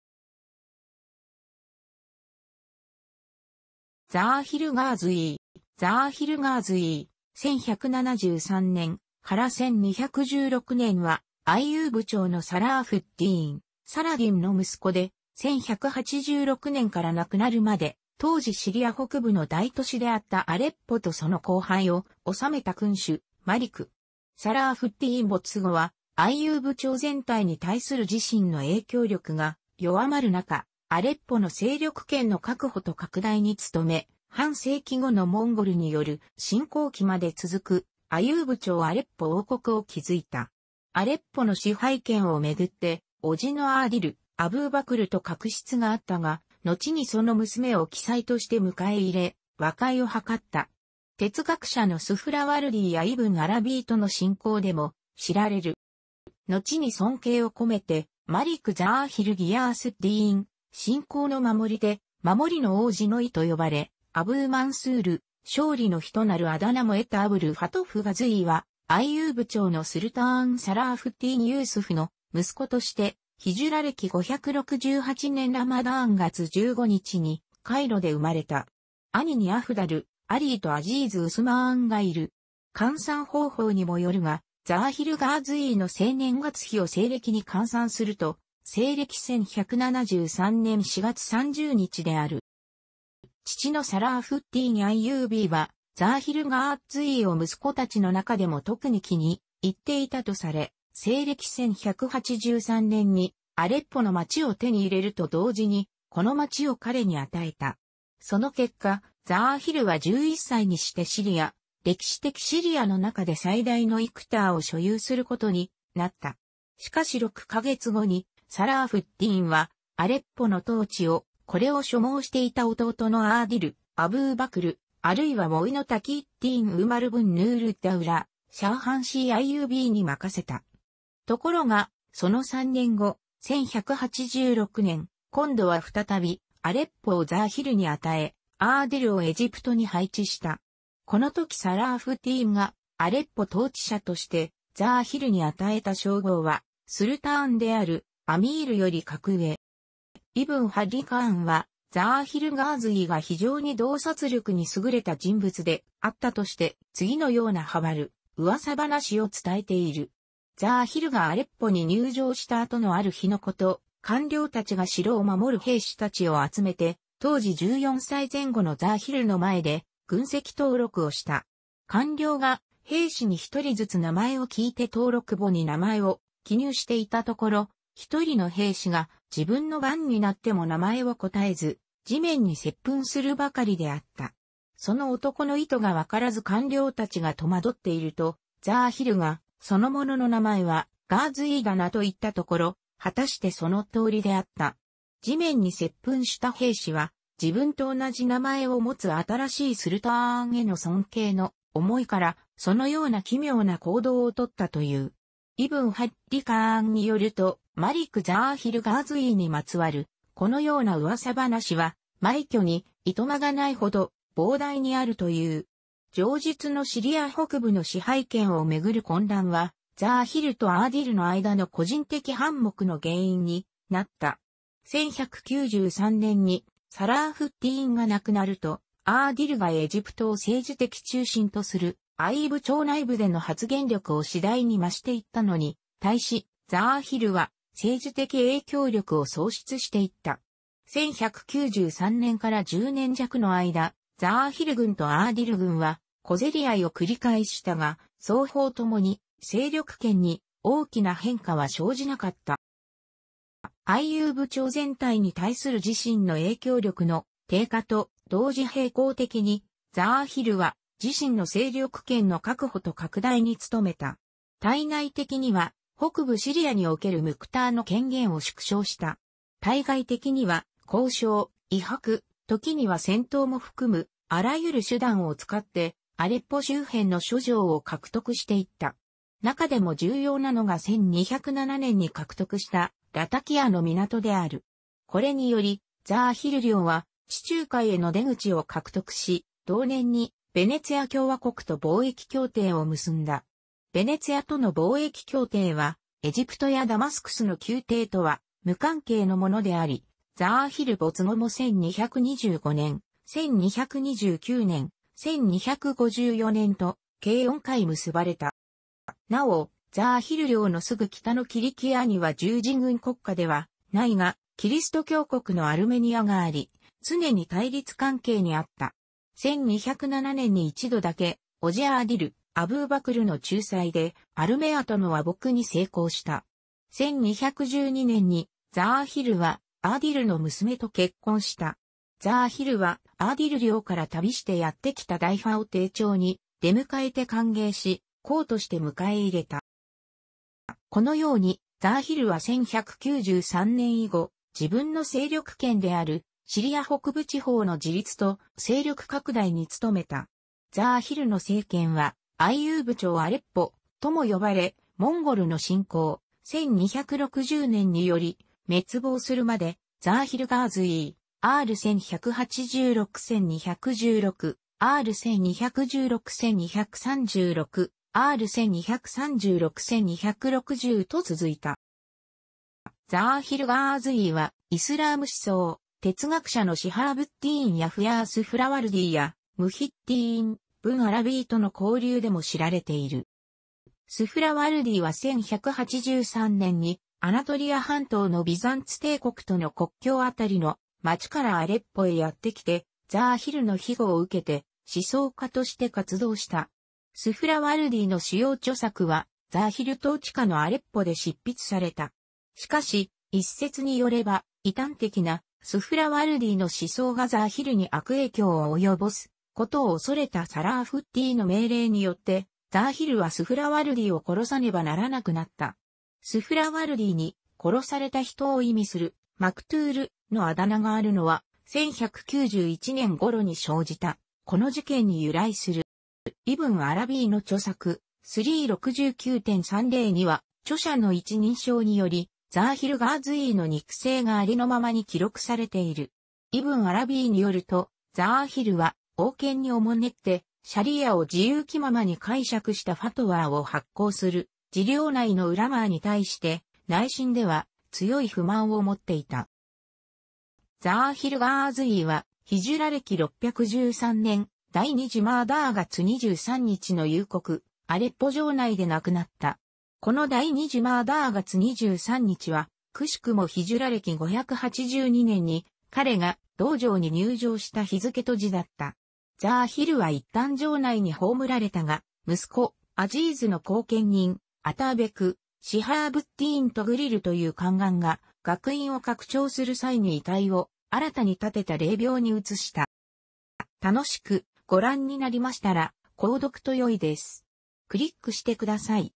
ザーヒル・ガーズィー
ザーヒル・ガーズィー（、1173年 - 1216年）は、アイユーブ朝のサラーフッディーン（サラディン）の息子で、1186年から亡くなるまで、当時シリア北部の大都市であったアレッポとその後背地を治めた君主（マリク）。サラーフッディーン没後はアイユーブ朝全体に対する自身の影響力が弱まる中、アレッポの勢力圏の確保と拡大に努め、半世紀後のモンゴルによる侵攻期まで続く「アイユーブ朝アレッポ」王国を築いた。アレッポの支配権をめぐって叔父のアーディル・アブー・バクルと確執があったが、のちにその娘を后として迎え入れ、和解を図った。哲学者のスフラワルディーやイブン・アラビーとの親交でも知られる。

のちに尊敬を込めて「マリク・ザーヒル・ギヤースッディーン」（「信仰の守り手、護りの王子」の意）と呼ばれ、「アブー・マンスール」（勝利の人）なるあだ名も得たアブル・ファトフ・ガーズィーは、アイユーブ朝のスルターン・サラーフッディーン・ユースフの息子として、ヒジュラ暦568年ラマダーン月15日にカイロで生まれた。兄にアフダル・アリーとアズィーズ・ウスマーンがいる。換算方法にもよるが、ザーヒル・ガーズィーの生年月日を西暦に換算すると西暦1173年4月30日である。

父のサラーフッディーン・アイユービーは、ザーヒル・ガーズィーを息子たちの中でも特に気に入っていたとされ、西暦1183年にアレッポの町を手に入れると同時に、この町を彼に与えた。その結果、ザーヒルは11歳にしてシリア（歴史的シリア）の中で最大のイクターを所有することになった。しかし6ヵ月後にサラーフッディーンはアレッポの統治を、これを所望していた弟のアーディル・アブー・バクル（あるいは甥のタキーッディーン・ウマル・ブン・ヌールッダウラ・シャーハンシャー・アイユービー）に任せた。ところが、その3年後（1186年）、今度は再びアレッポをザーヒルに与え、アーディルをエジプトに配置した。このときサラーフッディーンがアレッポ統治者としてザーヒルに与えた称号は「スルターン」である（アミールより格上）。

イブン・ハッリカーンはザーヒル・ガーズィーが非常に洞察力にすぐれた人物であったとして、次のようなハバル（噂話）を伝えている。ザーヒルがアレッポに入城した後のある日のこと、官僚たちが城を守る兵士たちを集めて当時14歳前後のザーヒルの前で軍籍登録をした。官僚が兵士に一人ずつ名前を聞いて登録簿に名前を記入していたところ、一人の兵士が自分の番になっても名前を答えず、地面に接吻するばかりであった。その男の意図がわからず官僚たちが戸惑っていると、ザーヒルが「その者の名前はガーズィーだな」と言ったところ、はたしてその通りであった。地面に接吻した兵士は、自分と同じ名前を持つ新しいスルターンへの尊敬の思いから、そのような奇妙な行動をとったという。イブン・ハッリカーンによると、マリク・ザーヒル・ガーズィーにまつわるこのような噂話は、枚挙にいとまがないほど膨大にあるという。
上述のシリア北部の支配権をめぐる混乱は、ザーヒルとアーディルの間の個人的反目の原因になった。1193年にサラーフッディーンが亡くなると、アーディルがエジプトを政治的中心とするアイユーブ朝内部での発言力を次第に増していったのに対し、ザーヒルは政治的影響力を喪失していった。1193年から10年弱の間、ザーヒル軍とアーディル軍は小競り合いを繰り返したが、双方ともに勢力圏に大きな変化は生じなかった。

アイユーブ朝全体に対する自身の影響力の低下と同時並行的にザーヒルは、自身の勢力圏の確保と拡大に努めた。対内的には北部シリアにおけるムクターの権限を縮小した。対外的には、交渉、威迫、ときには戦闘も含むあらゆる手段を使ってアレッポ周辺の諸城を獲得していった。中でも重要なのが1207年に獲得したラタキアの港である。これによりザーヒル領は地中海への出口を獲得し、同年にヴェネツィア共和国と貿易協定を結んだ。ヴェネツィアとの貿易協定はエジプトやダマスクスの宮廷とは無関係のものであり、ザーヒル没後も1225年、1229年、1254年と、計4回結ばれた。

なお、ザーヒル領のすぐ北のキリキアには十字軍国家ではないがキリスト教国のアルメニアがあり、常に対立関係にあった。1207年に一度だけ、叔父アーディル・アブー・バクルの仲裁でアルメニアとの和睦に成功した。1212年にザーヒルは、アーディルの娘と結婚した。ザー...